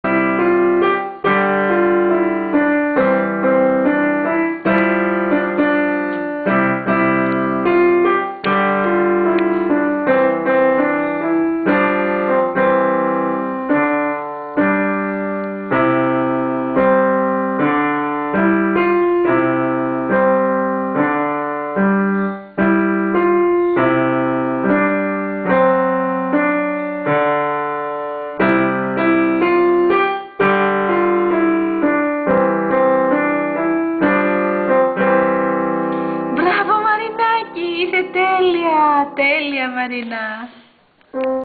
2010piano.mpg
Marina is learning the piano and shows great promise!